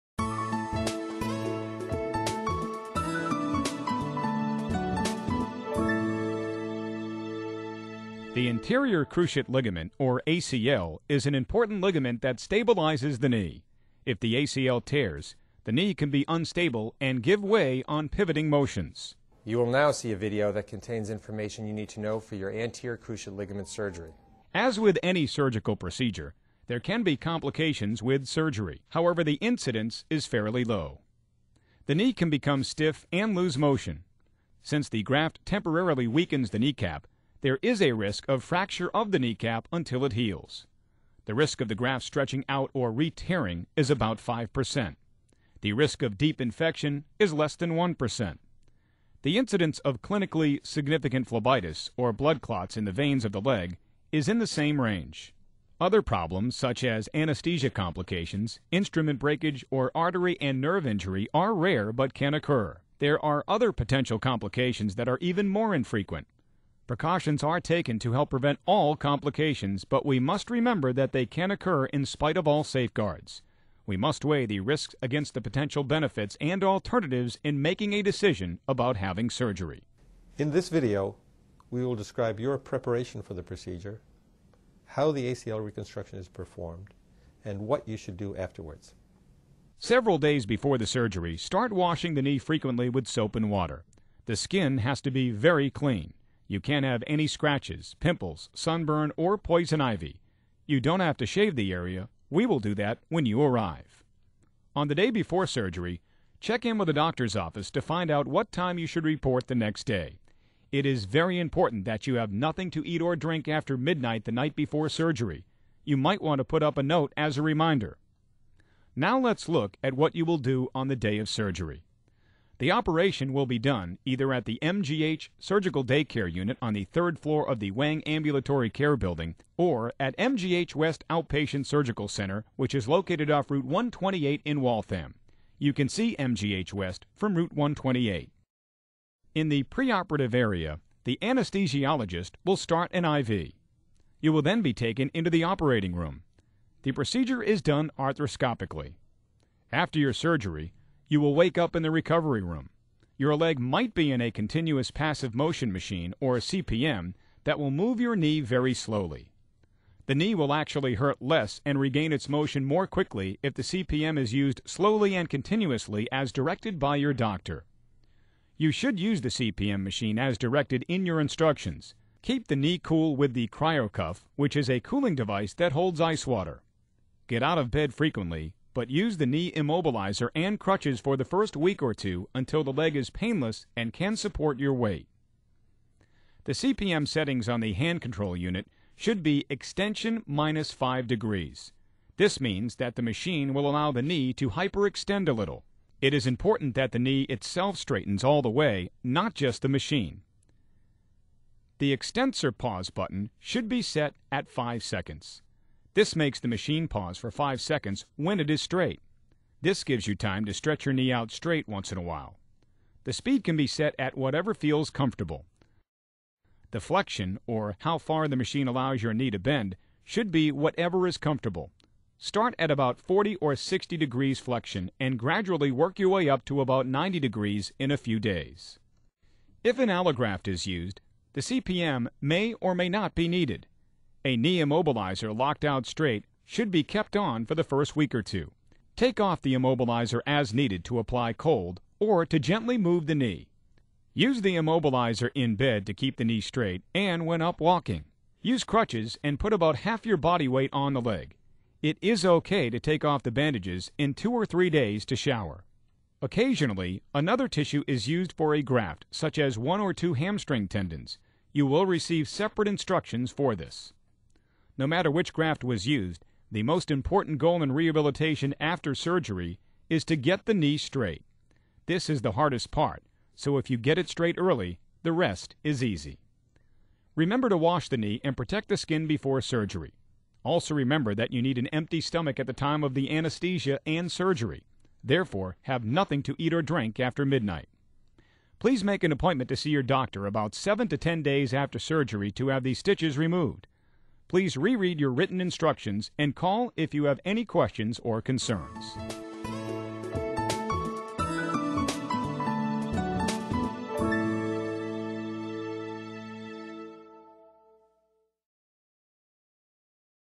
Patient Info on ACL Reconstruction Surgery at MGH
Dr. Bertram Zarins has prepared this video to educate and inform patients on what they need to know before having Anterior Cruciate Ligament (ACL) Reconstruction surgery at the Mass General Hospital Sports Medicine Service.  for more information.